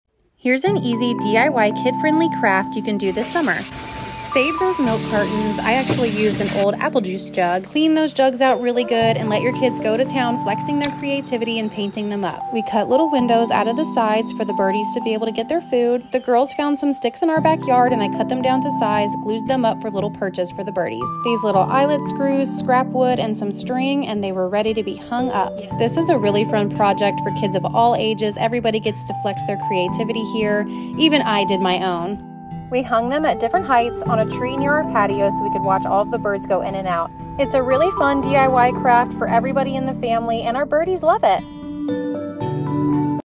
Easy DIY Kid Friendly Craft at home, a bird feeder! Using recyclables.
Easy DIY kid crafts to do at home are HUGE on my summer to-do list this year. There are so many fun things that can be created out of daily recyclables, I’m looking forward to sharing some of my favorites over the next few weeks. A DIY bird feeder was called for as the season was getting warmer and our backyard has been filled with happy morning chirps, we wanted to give our bird friends a cool hang out spot.

All you need is:
- milk jug or juice jug
- paint (we mostly used outdoor acrylics)
- Scissors
- Hot glue
- Sticks from the yard
- Stem cutters (or something sturdy enough to cut sticks)
- Eye hook
- Twine
- Scrap wood planks (@dollartree also has plain planks)

It is as simple as clean the jug, painting the jug to your liking, cutting the windows, glueing the sticks how you want (our bird friends like to perch on them), hang the whole thing on the wood plank, and set in a tree!

Will you try this easy DIY kids project this summer?

Share with someone who enjoys a fun and easy DIY craft!